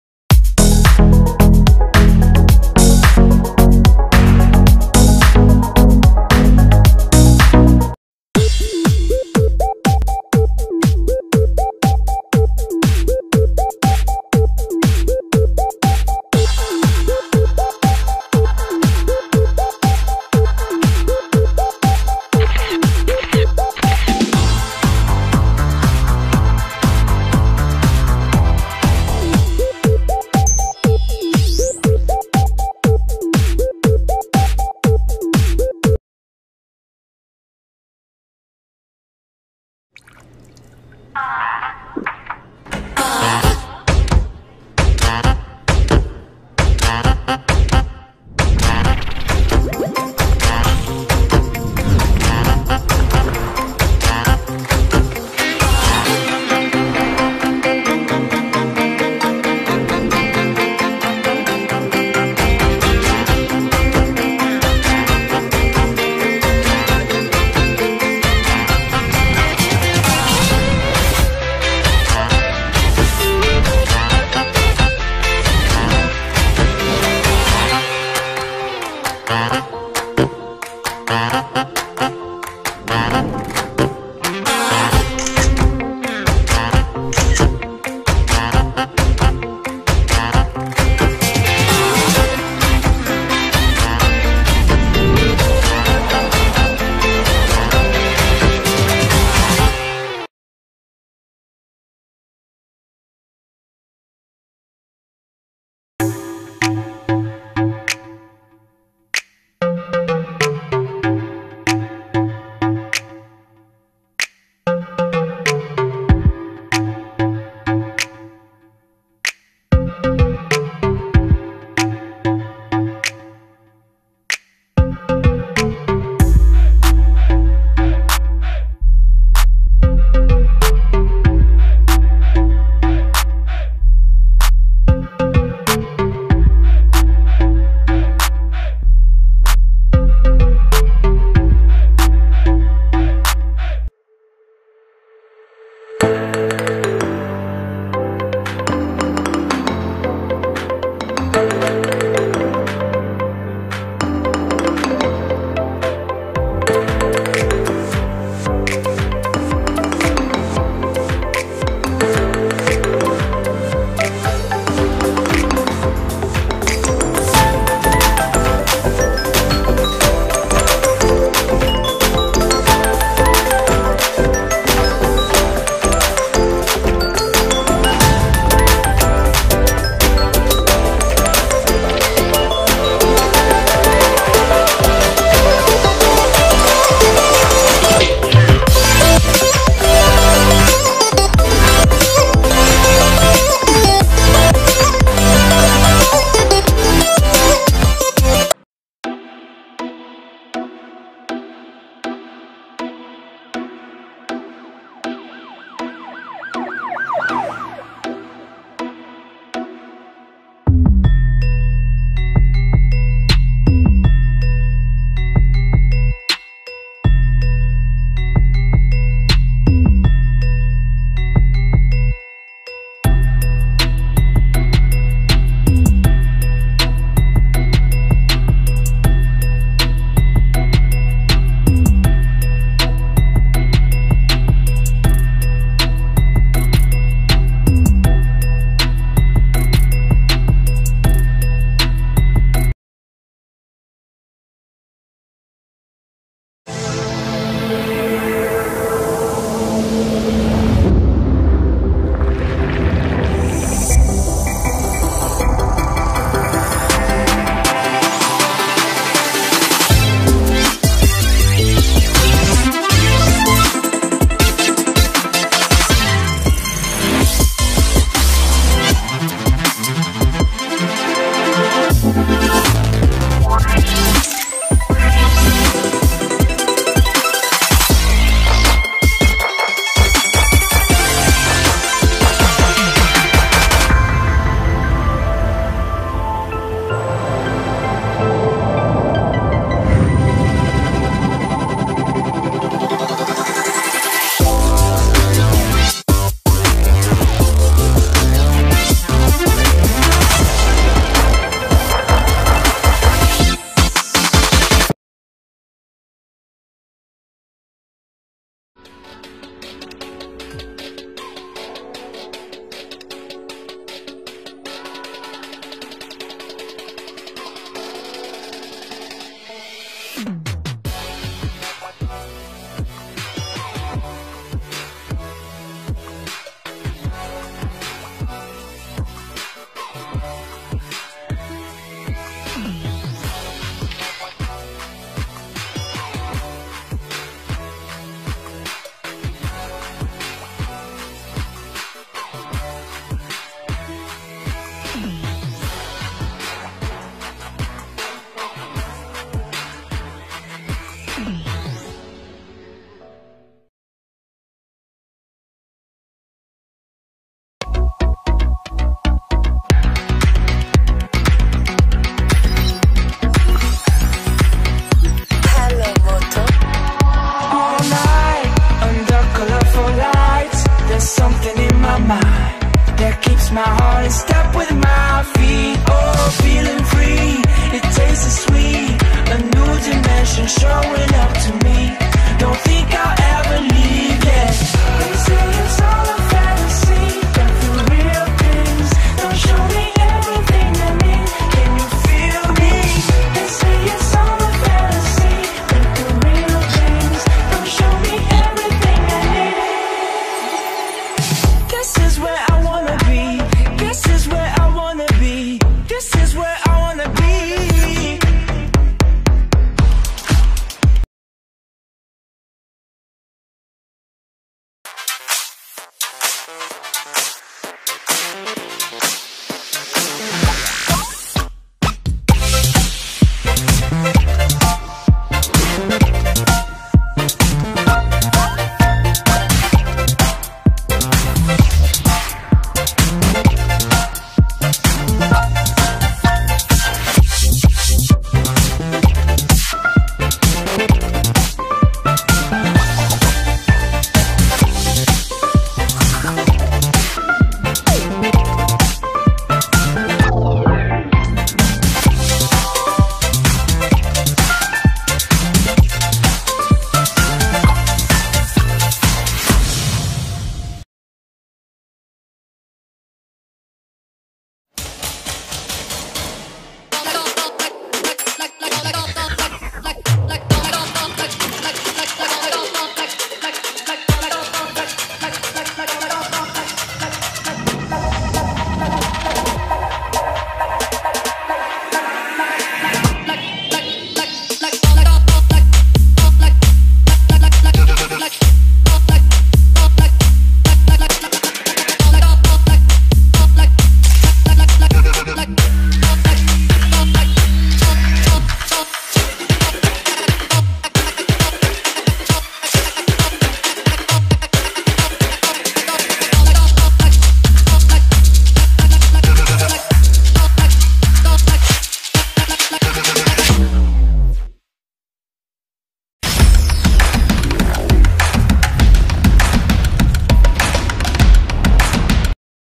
Top best Mid-Range Smartphones | with Amazing features
If you are in the market looking for a budget Android smartphone, chances are, you have a budget of Rs. 10,000. So, what do you get in this budget? well, a lot actually. As technology continues to become cheaper with each passing month, features such as 13MP rear cameras, Full HD+ displays, fingerprint sensors have started to make their way in this segment. But, which phone to buy?. Here is our list of the top Android phone in 10000 in India for this month across different brands. These are the best android phones under 10000 and offer the best combination of features, performance and quality for a phone buyer whose budget is under Rs 10,000

THANKS!

1)This video has no negative impact on the original works (It would actually be positive for them)
2) This video is also for Good purposes.